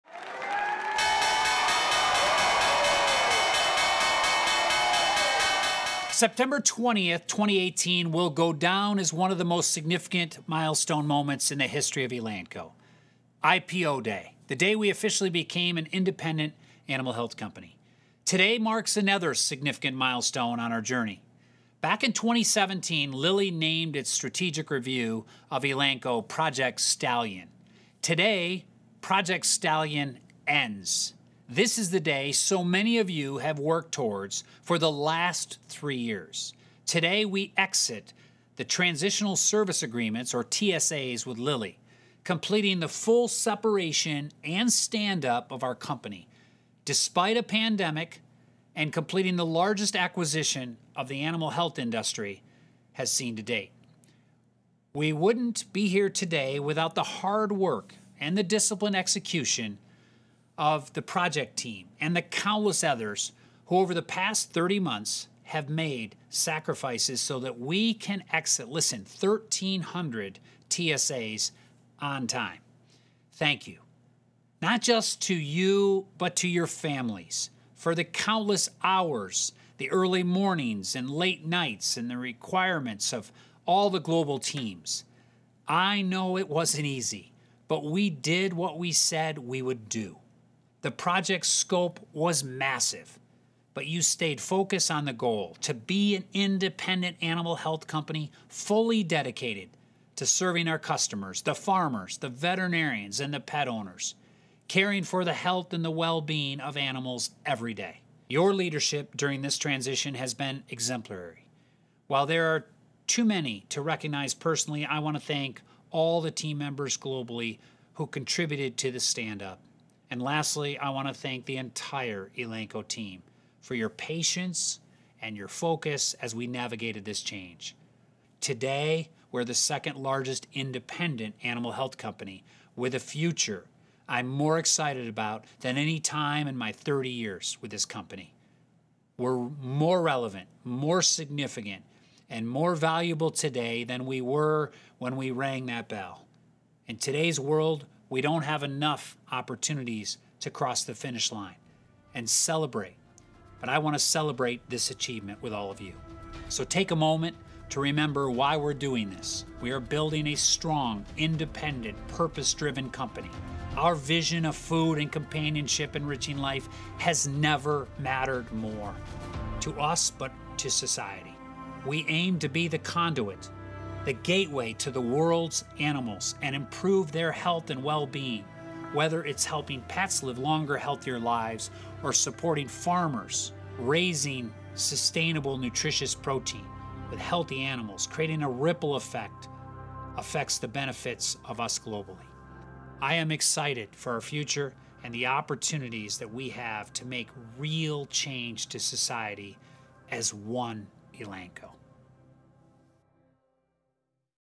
Elanco Celebrates Complete Separation from Lilly
This is the day so many of you have worked toward for the last THREE YEARS - today we exit the Transitional Service Agreements or TSAs with Lilly, completing the full separation and stand up of our company despite a pandemic and completing the largest acquisition the Animal Health industry has seen to date. We wouldn’t be here today, without the hard work and disciplined execution of the project team and countless others who over the past 30 months, have made sacrifices so that we can exit 1300 TSAs on time. Thank you.

Our journey isn’t quite over, but let’s take the time and appreciate this significant milestone today. Three years ago we were fighting to gain our right to be independent and planning for the potential of this day. Today, we’re the second-largest independent animal health company with a future I'm more excited about than at any time in my 30 years. We’re more relevant, more significant, and more valuable today than we were when we rang that bell.  In today’s world, we don’t have enough opportunities to cross the finish line. Celebrate this achievement.